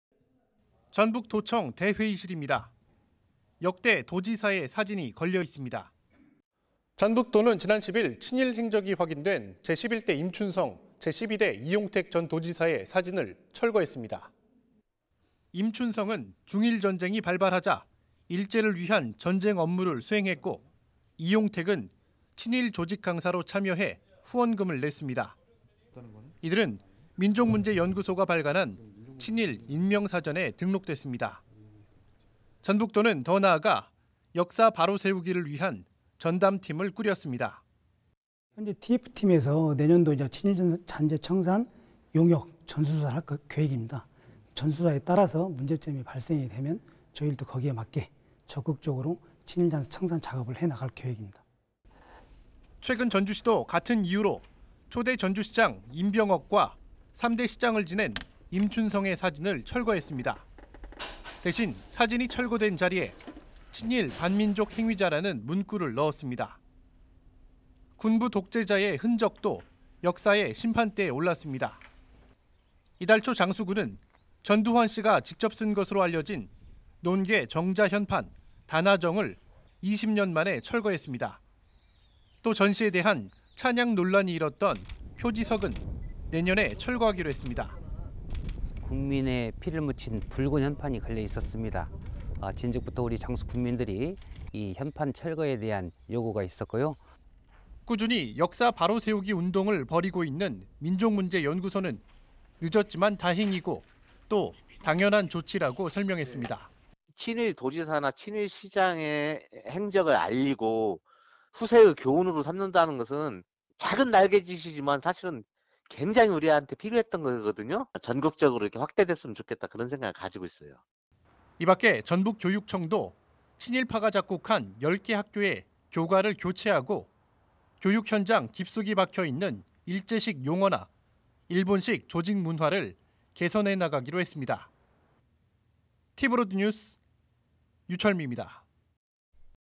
"늦었지만 당연"…'역사 바로 세우기' 활발
'역사를 잊은 민족에게 미래는 없다' 단재 신채호 선생이 남긴 유명한 말이죠.
최근 전북 곳곳에서 역사 바로 세우기 움직임이 본격화하고 있습니다.